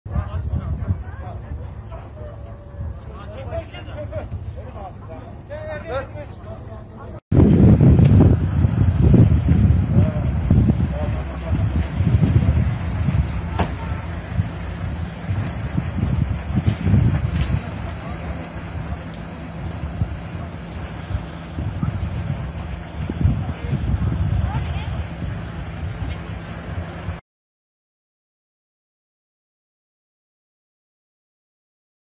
Polis memuru Serhat Alışkan trafik kazasında şehit oldu!
Kütahya'nın Tavşanlı ilçesindeki trafik kazasında ağır yaralanan polis memuru Serhat Alışkan, kaldırıldığı hastanede kurtarılamayarak şehit oldu.